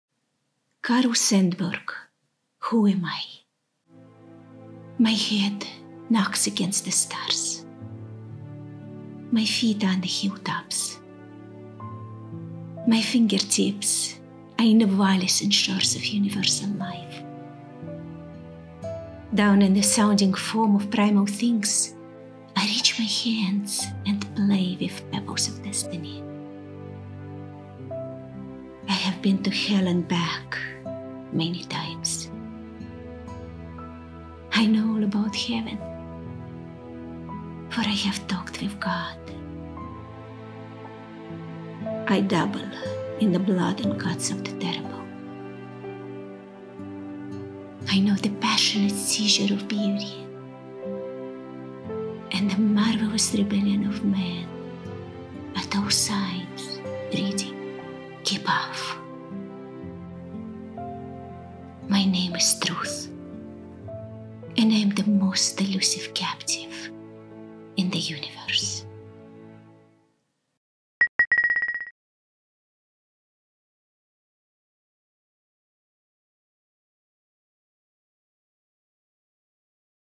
“Who am I?” by Carl Sandburg – motivational poetry
Truth seems to be in short supply these days… Though, perhaps, it was always the case. I love how Carl Sandburg describes it in his poem “Who am I?”.  Full text below.

Carl Sandburg (January 6, 1878 – July 22, 1967) was an American poet, biographer, journalist, and editor. He won three Pulitzer Prizes: two for his poetry and one for his biography of Abraham Lincoln. During his lifetime, Sandburg was widely regarded as "a major figure in contemporary literature", especially for volumes of his collected verse, including “Chicago Poems” (1916), “Cornhuskers” (1918), and “Smoke and Steel” (1920). He enjoyed "unrivaled appeal as a poet in his day, perhaps because the breadth of his experiences connected him with so many strands of American life", and at his death in 1967, President Lyndon B. Johnson observed that "Carl Sandburg was more than the voice of America, more than the poet of its strength and genius. He was America."

My head knocks against the stars.
My feet are on the hilltops.
My finger-tips are in the valleys and shores of universal life.
Down in the sounding foam of primal things I reach my hands and play with pebbles of destiny.
I have been to hell and back many times.
I know all about heaven, for I have talked with God.
I dabble in the blood and guts of the terrible.
I know the passionate seizure of beauty
And the marvelous rebellion of man at all signs reading "Keep Off."

My name is Truth and I am the most elusive captive in the universe.